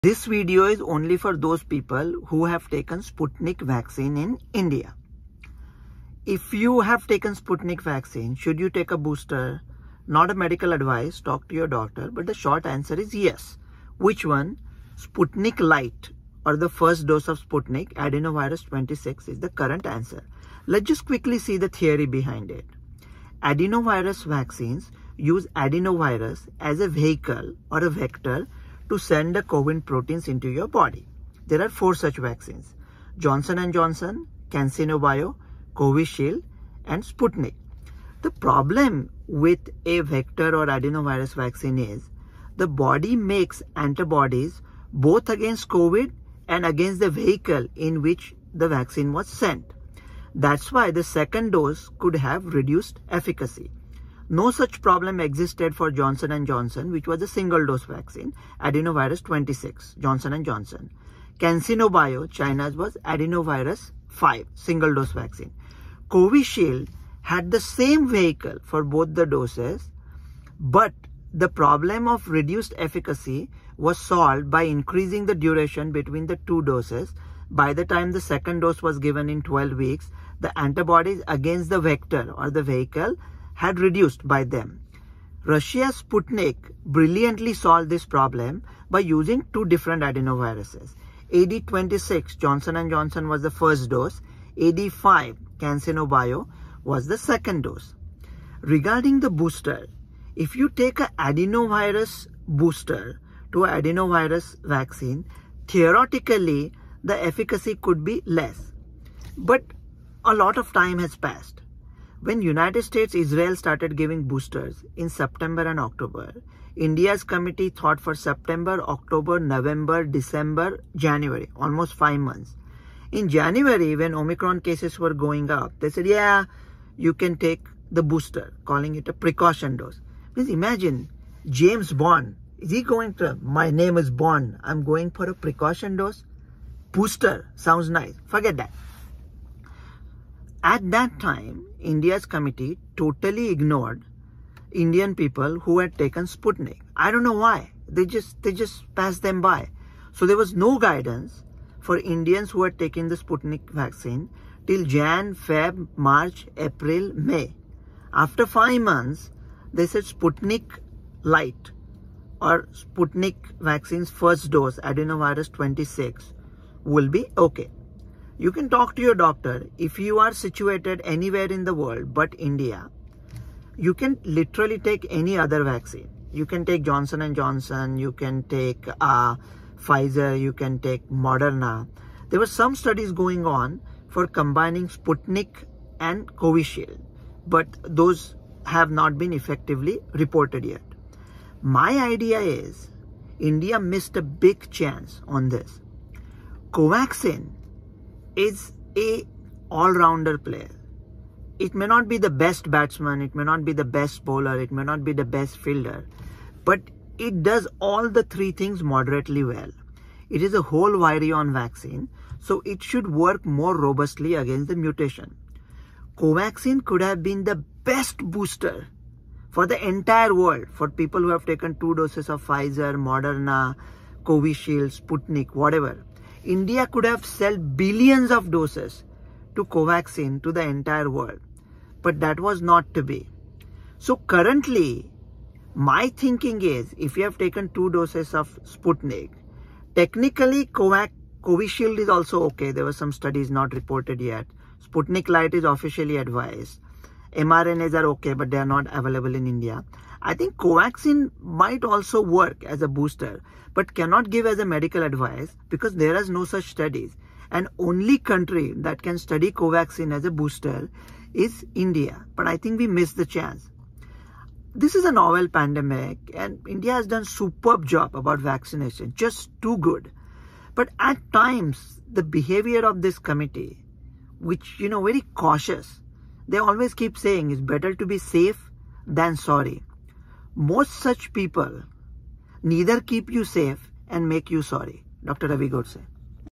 Which booster for Sputnik vaccine?
Sputnik light ( Ad 26) is currently recommended as a booster ( precaution dose) for those Indians who took the Sputnik vaccine series. This video briefly looks at the science of Adenovirus vaccines and makes some comments about Covaxin as a potential booster.